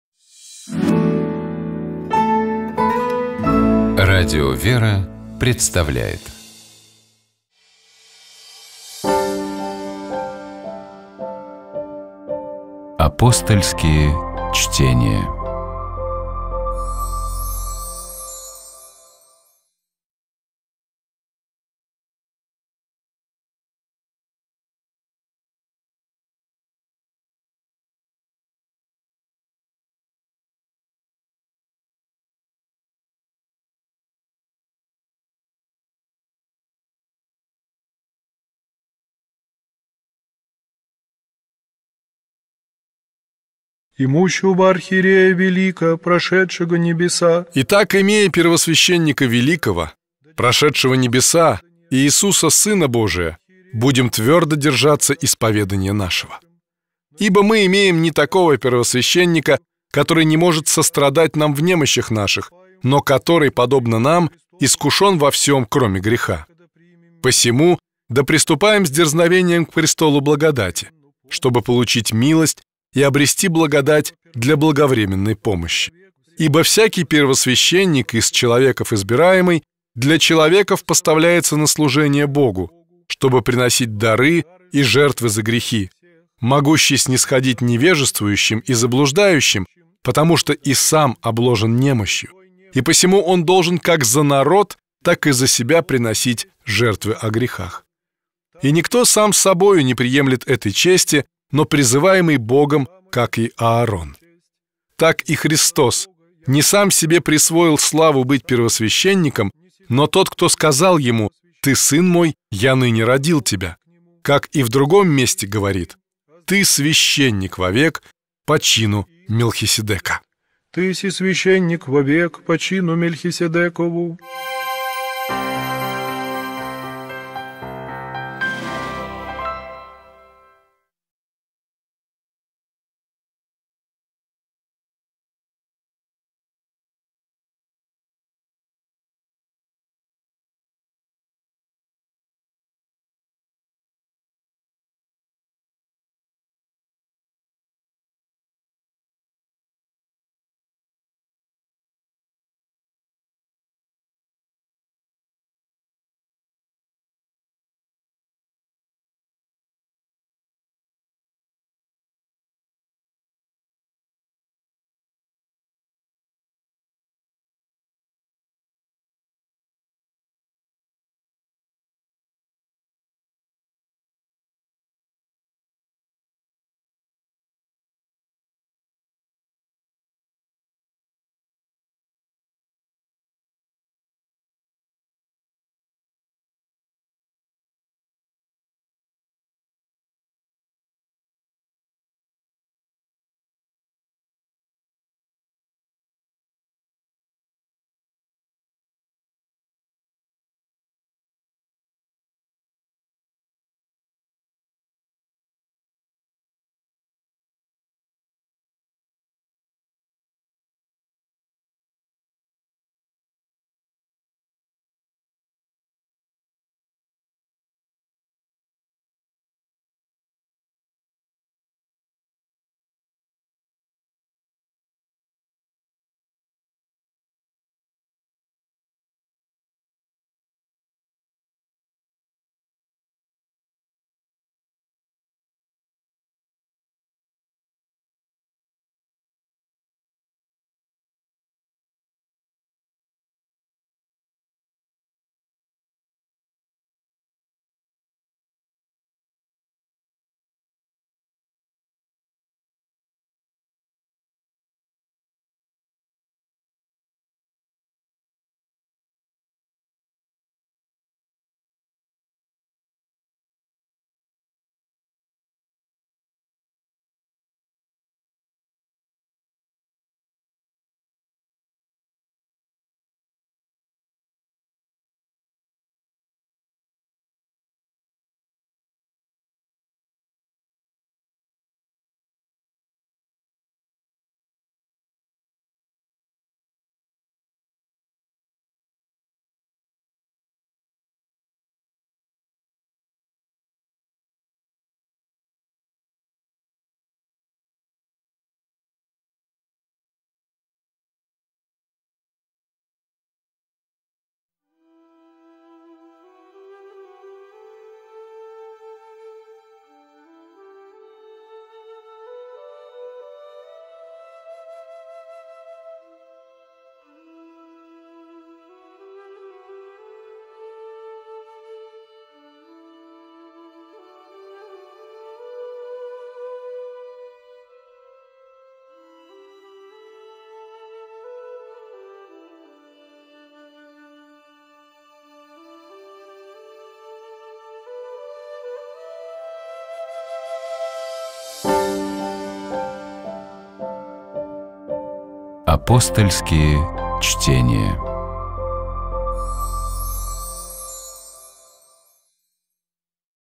Апостольские чтения. Христос - путь к Богу (Евр. IV: 14 - V: 6)
Комментирует священник Стефан Домусчи